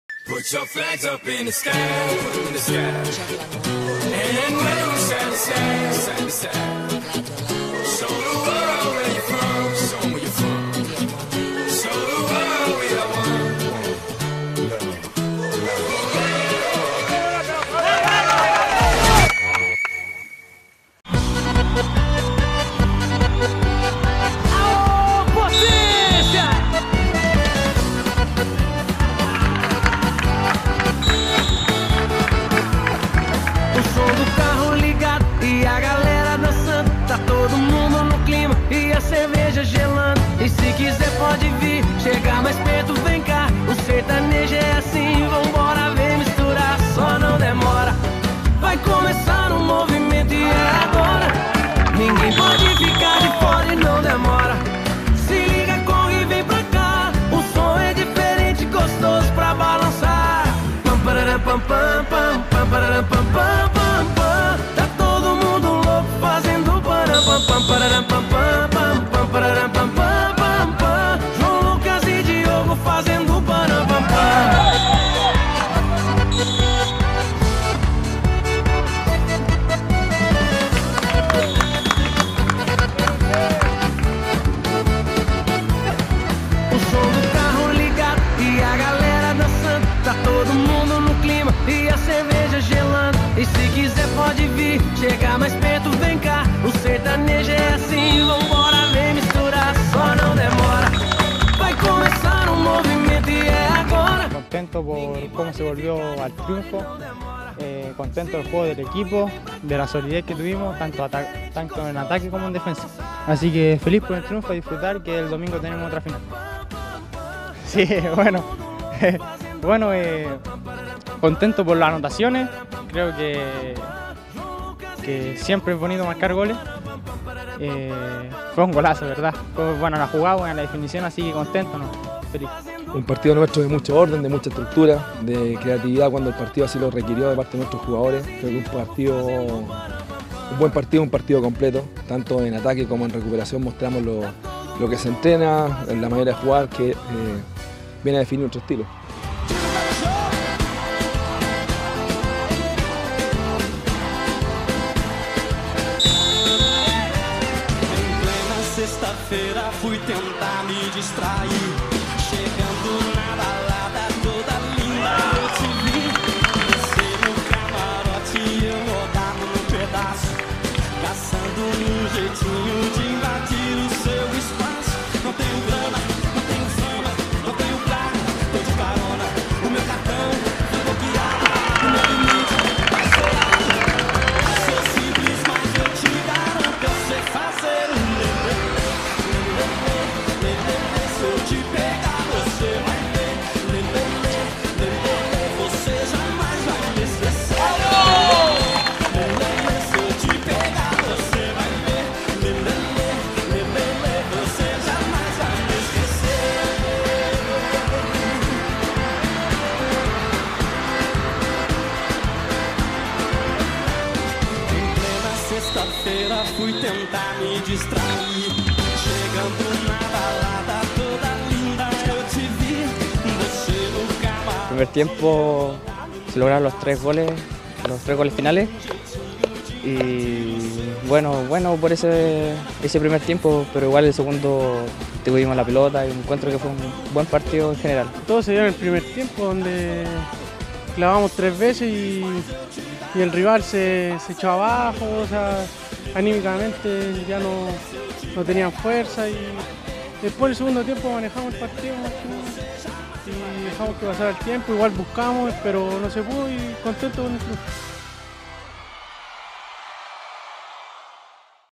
La 17 y la 19 arrasaron con O'Higgins
Ambos elencos derrotaron 3 a 0 a los rancagüinos.